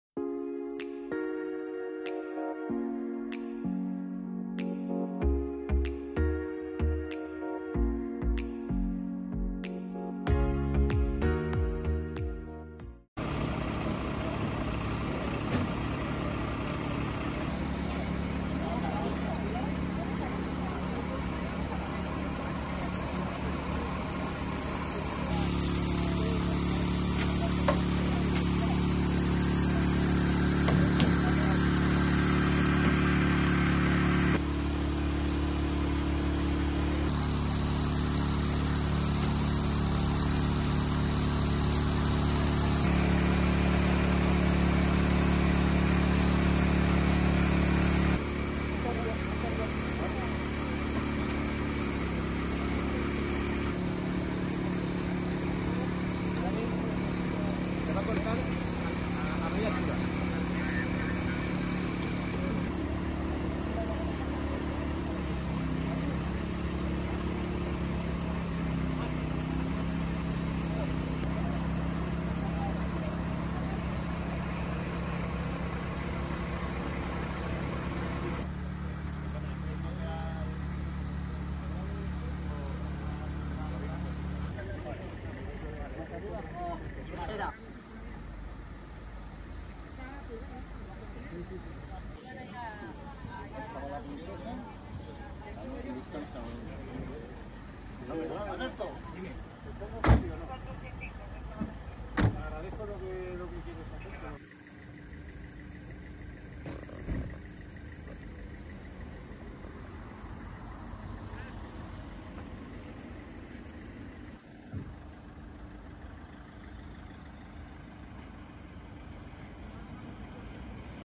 Cuatro jóvenes heridos al perder control del vehículo en una rotonda de Alcalá de Henares
6 de septiembre de 2023 / Cuatro jóvenes han resultado heridos  de diversa consideración en un accidente ocurrido esta noche  al estrellarse  su vehículo  contra el muro de la rotonda que separa La Garena de El Corte Inglés... /  Video del 112 publicado por @alcalahoy1